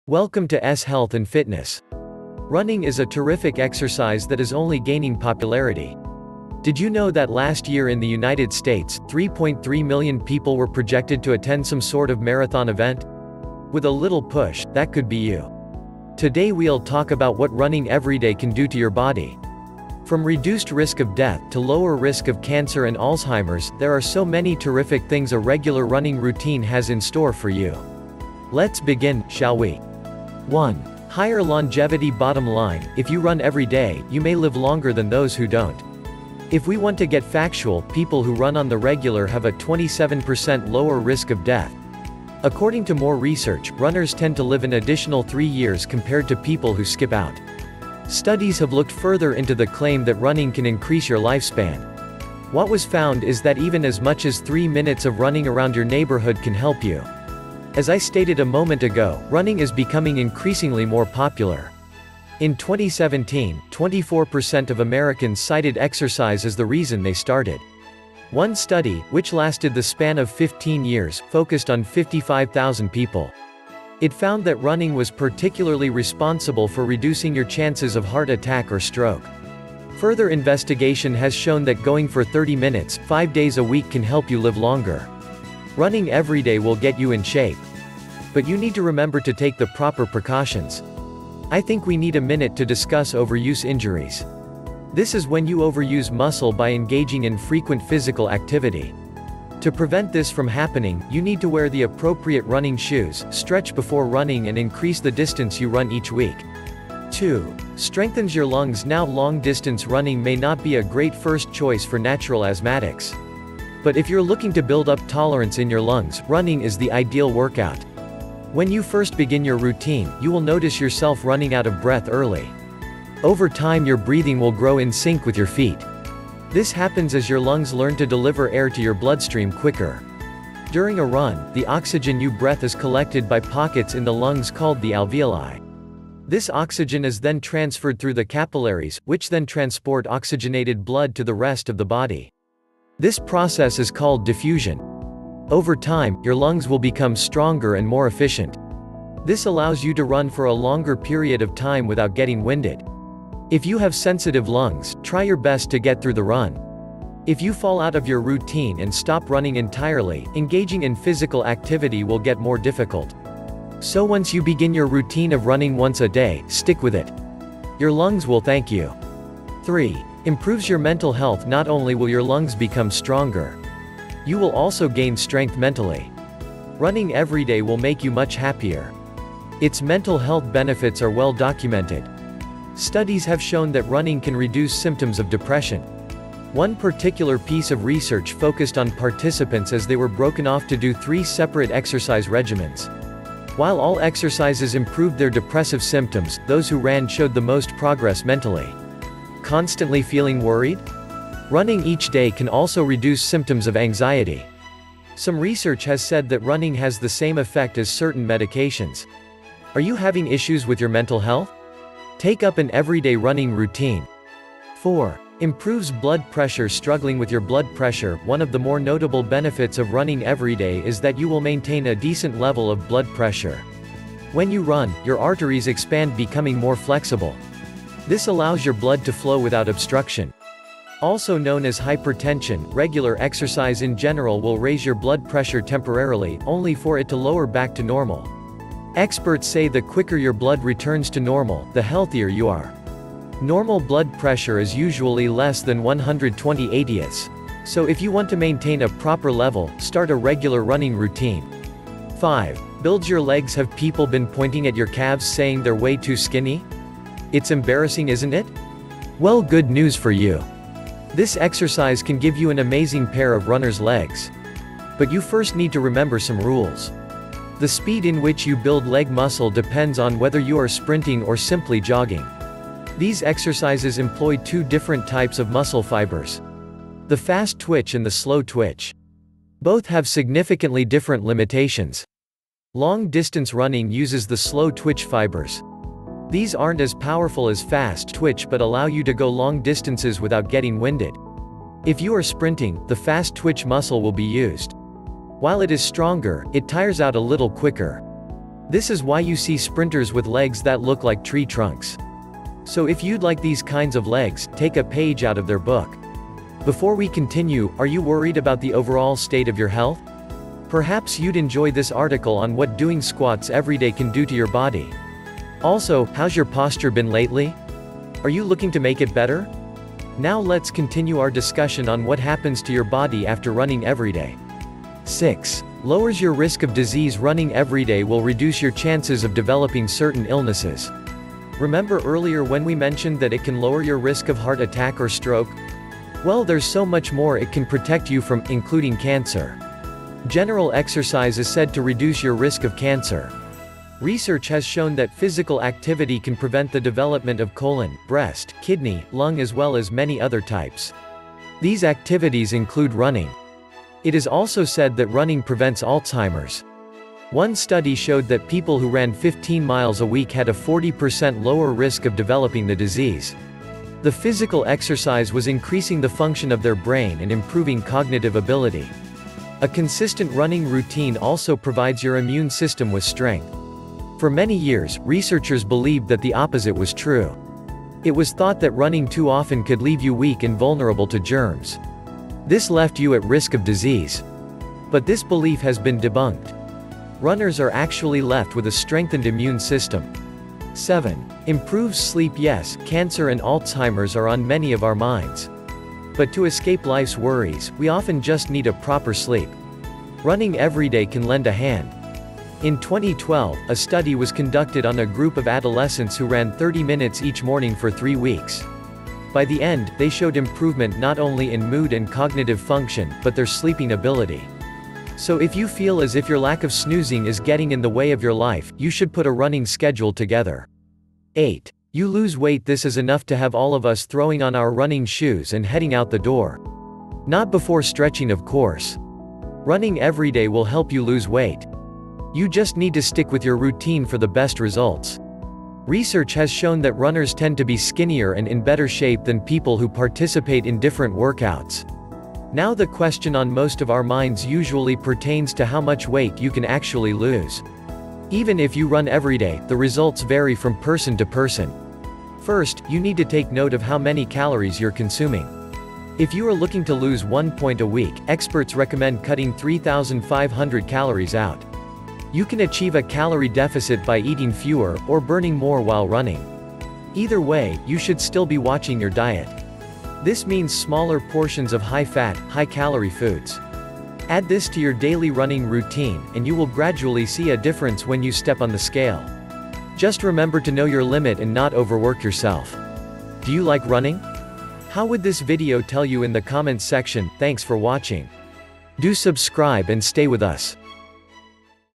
Run Every Day and See What Happens to Your Body
Hey Friends,
Welcome back to new video of S Health and fitness,
Today im going to tell you about Run Every Day and See What Happens to Your Body.